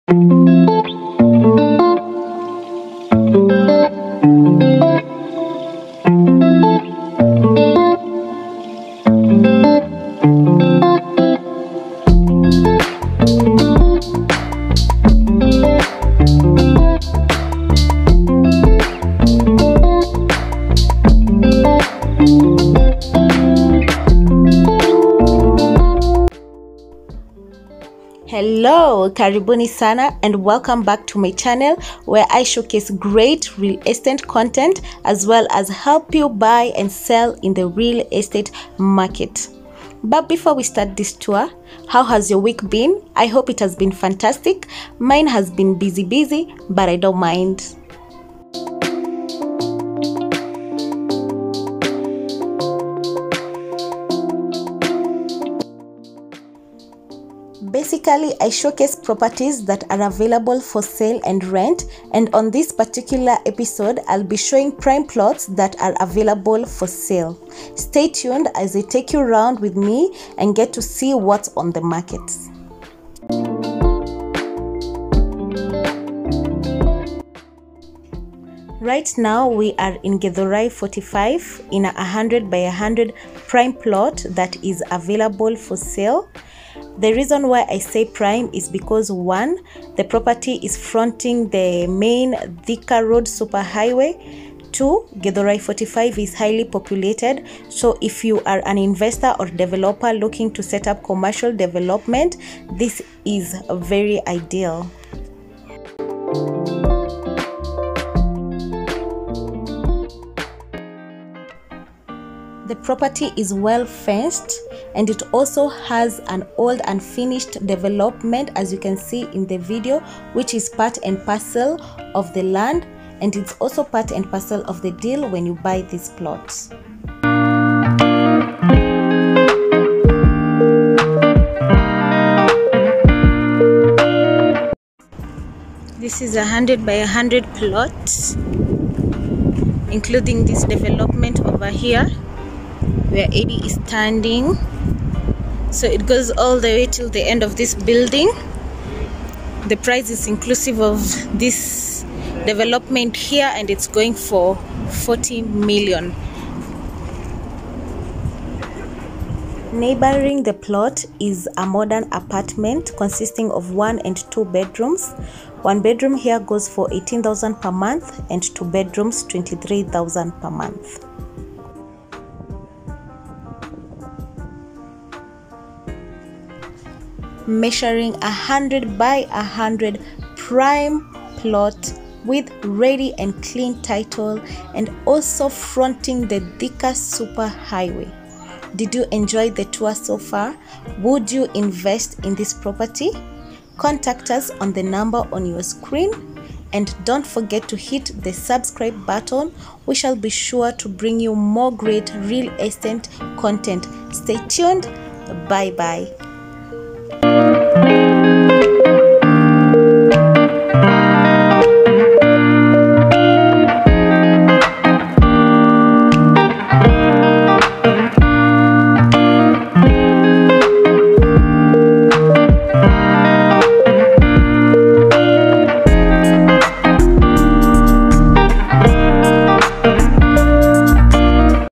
PRIME PLOTS FOR SALE IN NAIROBI KENYA /helping you buy & sell in Kenya
Prime plot for sale at Githurai 45 and fronting the Thika supper highway.
👉It's strategically located near the highway, and it's ideal for commercial development like rentals or a petrol station.
👉Measuring 100 by 100 plot with un unfinished development inside,
👉Ready clean Title,
👉Asking 40 Million
One bedroom in the area goes for 18k per month
And two bedrooms 23k per month.